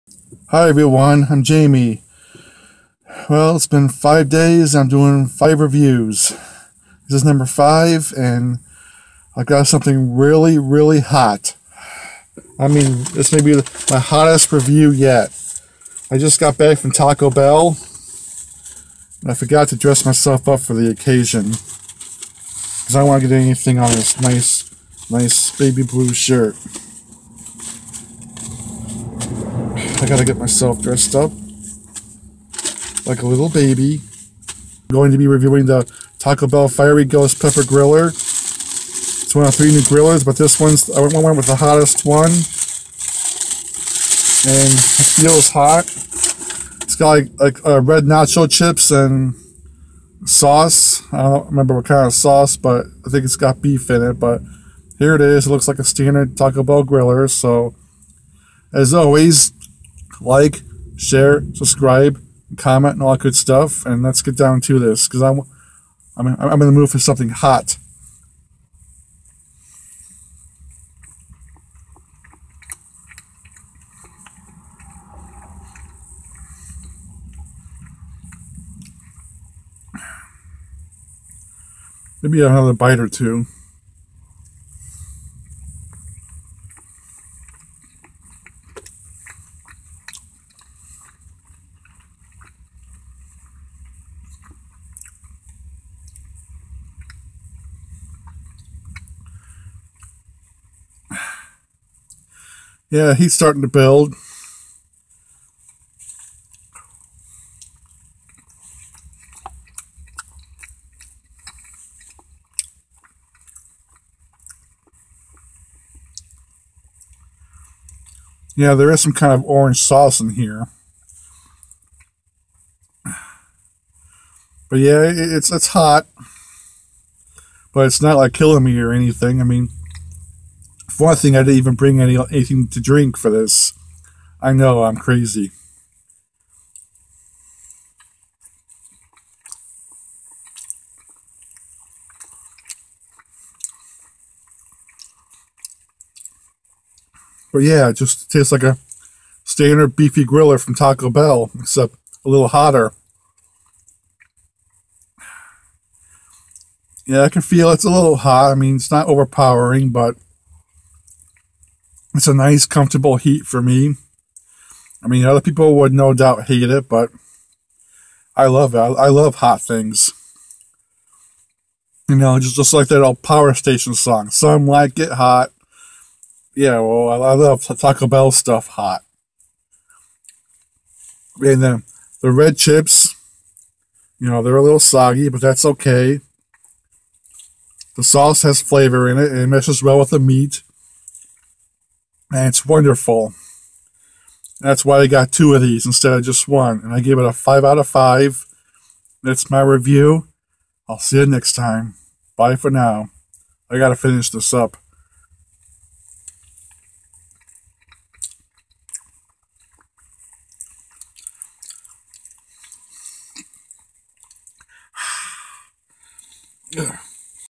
Jamie's Product Reviews/Taco Bell's Fiery Ghost Pepper Dare Devil Loaded Griller
Something new from Taco Bell for those who like it really HOT.